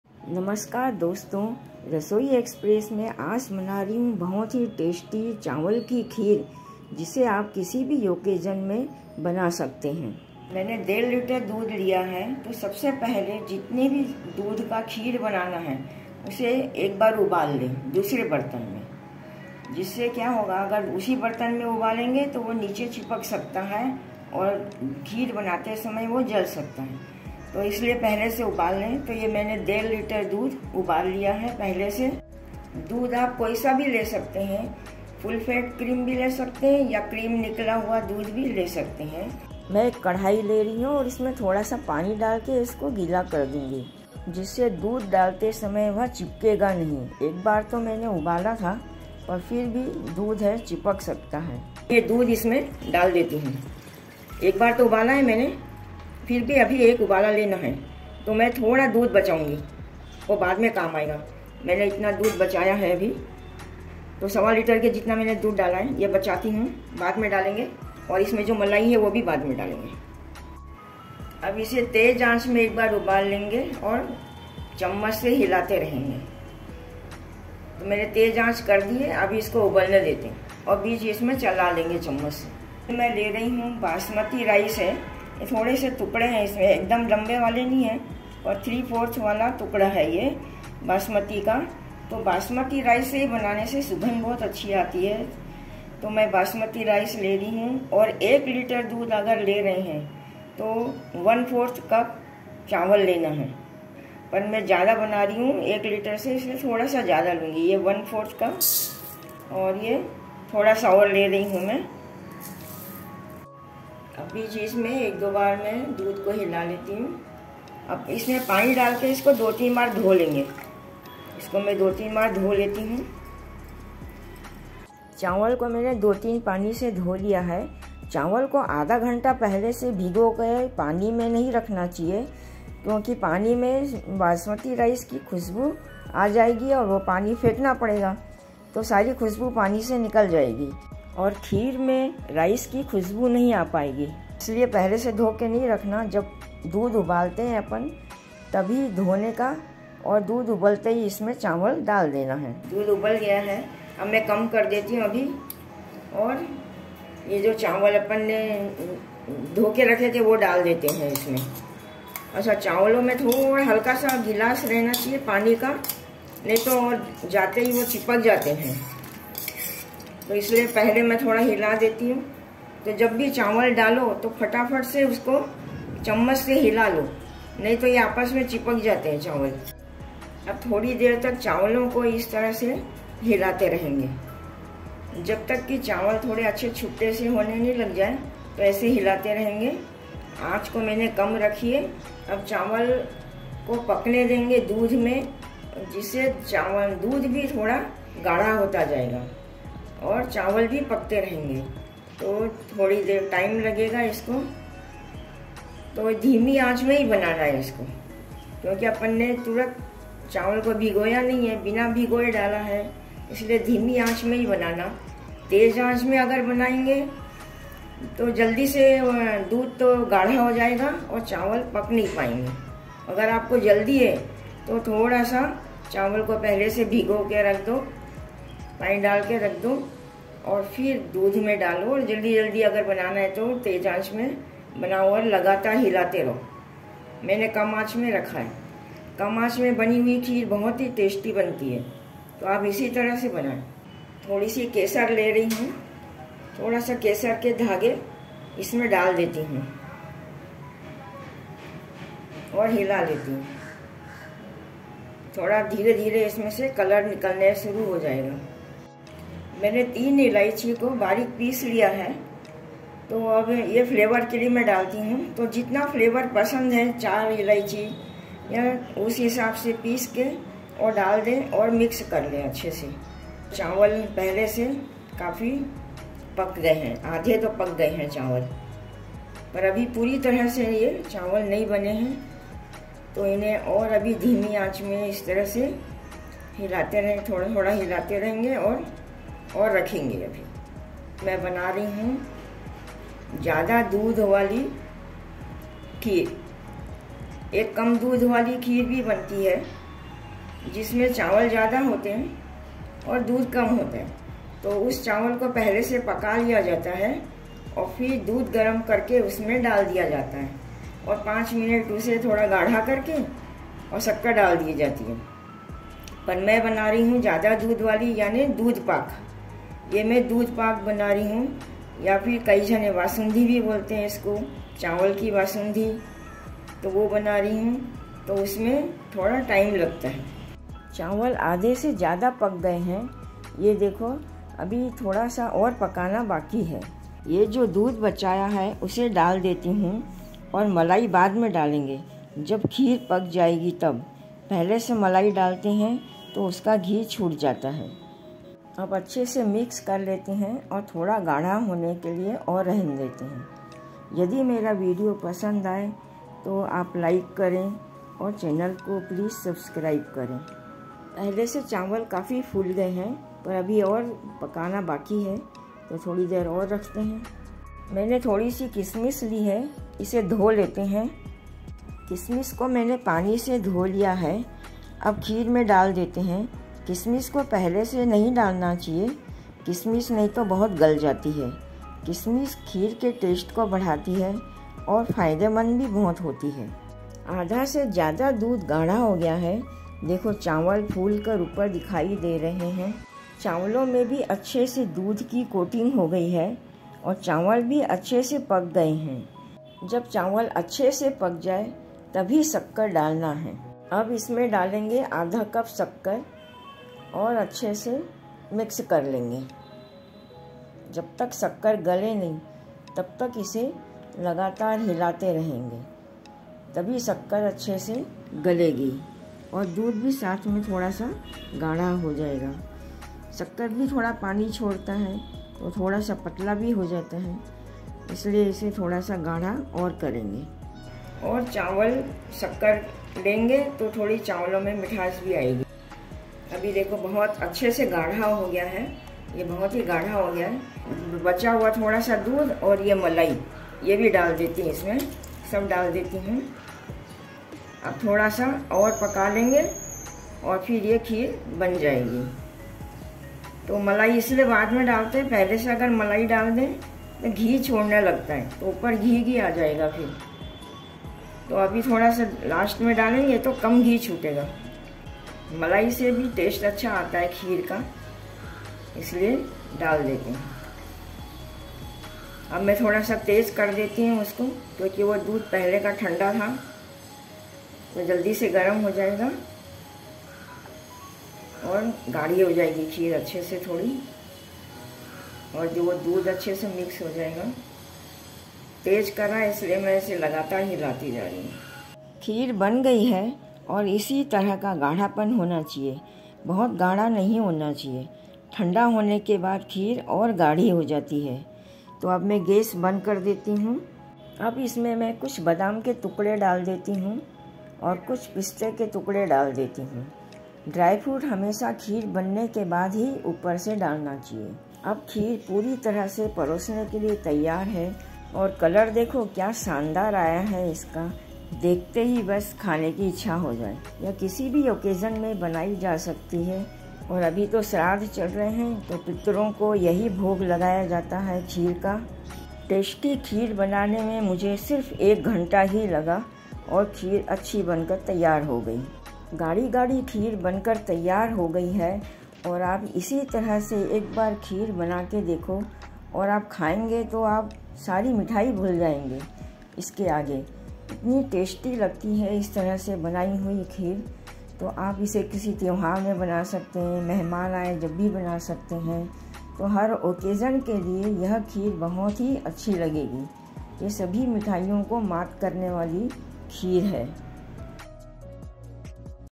रियल चावल की खीर इस तरह से बनती है |Riyal Rice Kheer Recipe | Chawal ki kheer banane ki vidhi
INGREDIENTS:-
Basmati rice 1/4 cup ( 50 gm)
Milk 1.5 lt
Sugar 1/2cup (100 gm)
Almond 6-7
     Pistachio
       Rasins
Green cardamoms 4
          Kesar

How to make kheer.  Rice kheer.
Chawal ki kheer. payasam recipe
Kheer banane ki recipe.

Musician: @iksonmusic